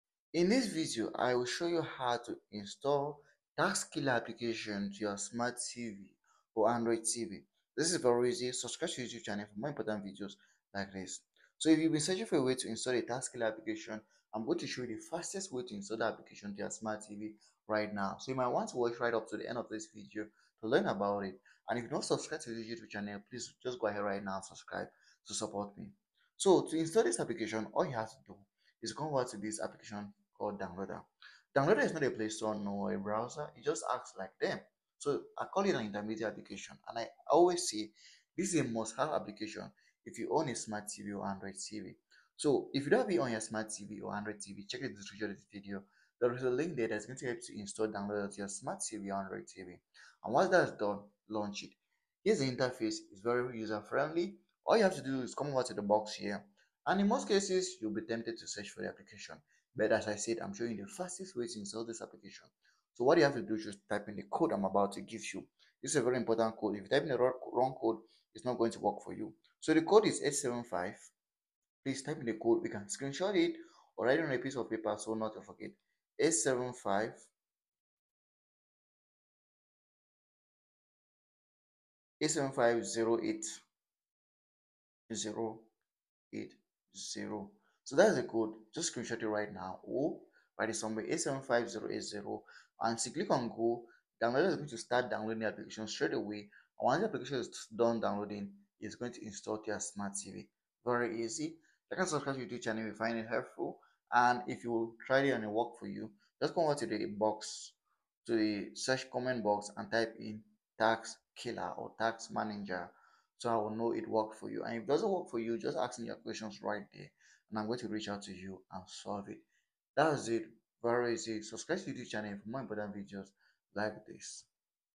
How to Install Task Killer App on Android TV or Smart TV in 2025 (Easiest Method)
You are on the right YouTube Channel if you are searching for How to Install Task Killer App on Android TV or Smart TV in 2025. Here, I will show you how to download and install any app on Smart TV or Android TV. In this particular video, I will show you the fasted method to Install Task Killer App on Android TV or Smart TV in 2025.